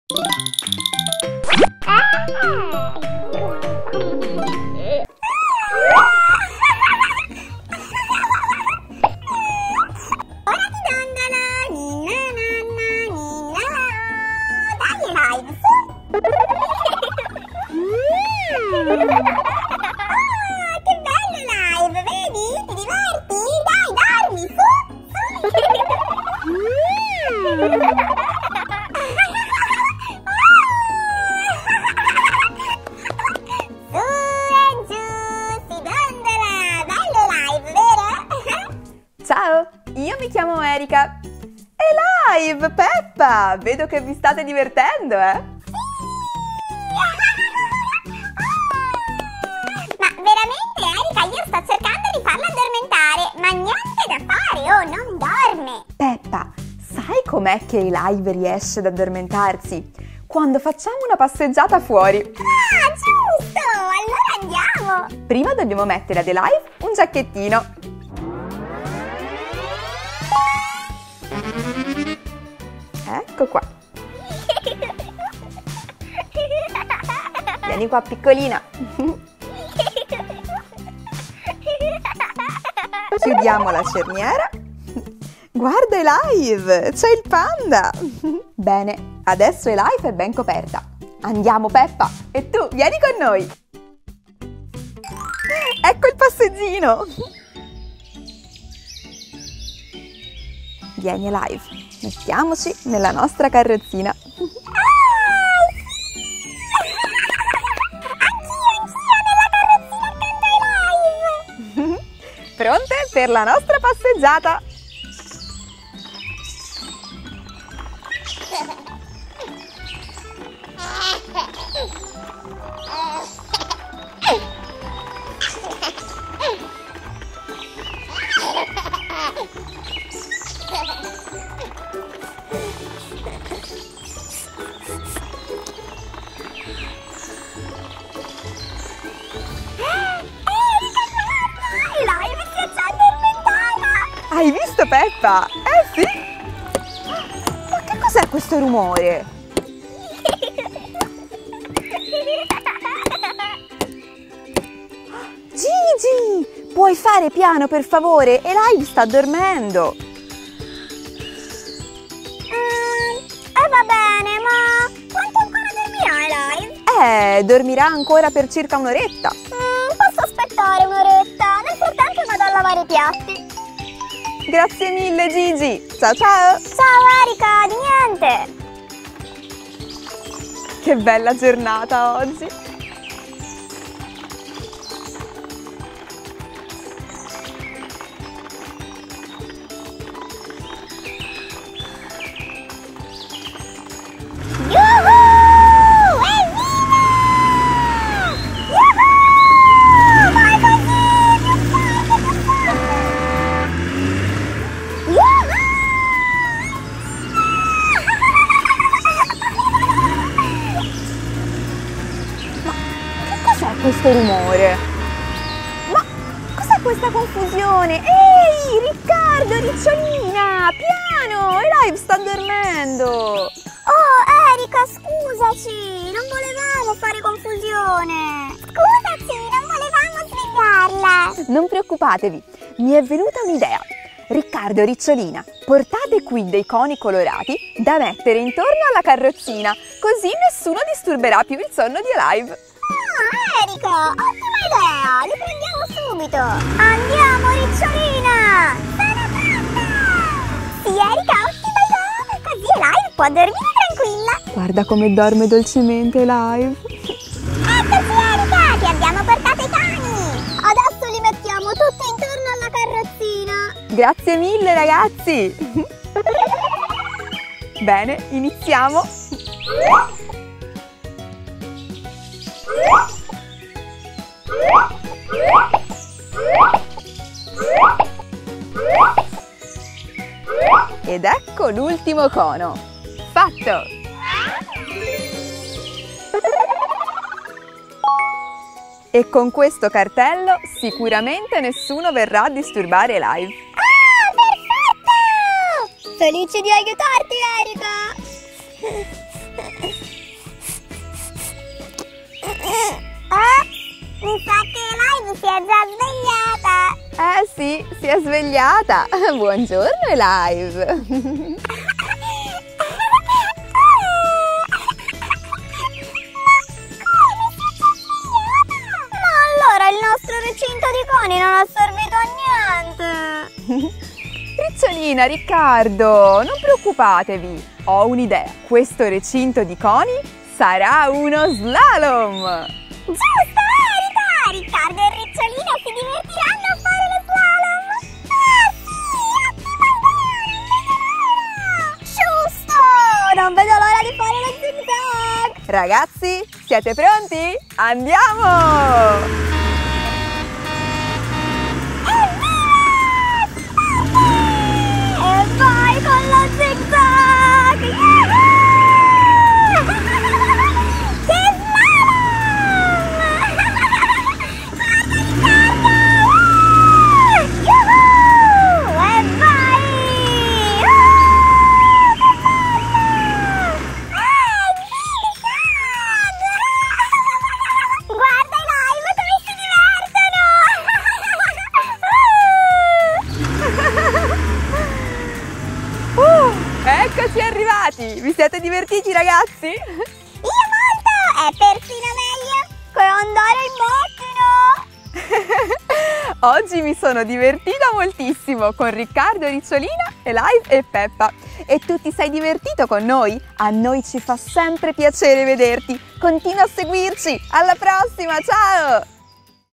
Una passeggiata per dormire bene! Giochi per bambini con la bambola Baby Alive e Peppa pig
La bambola Baby Alive oggi non riesce ad addormentarsi e quindi Peppa Pig decide di fare una passeggiata, così Alive può dormire bene! Guarda i nuovi giochi per bambini e partecipa anche tu, è molto divertente!